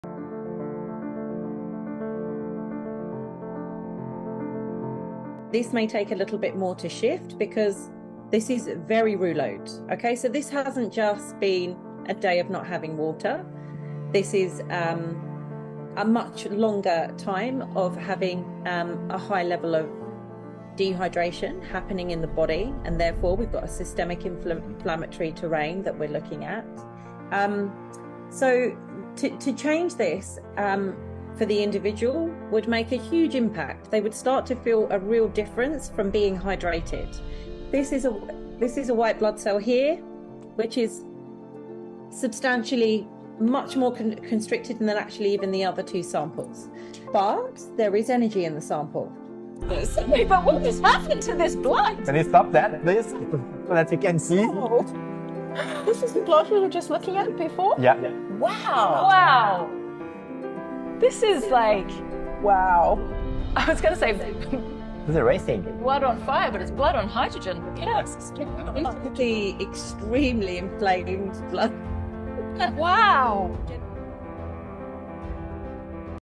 Muestra de sangre en microscopio con LV
Una pequeña muestra en microscopio de lo que pasa en tu sangre en tiempo real al usar la mejor tecnología para generar agua con hidrógeno molecular y frecuencias que cambian la deshidratación crónica y actúa en tiempo real a nivel celular.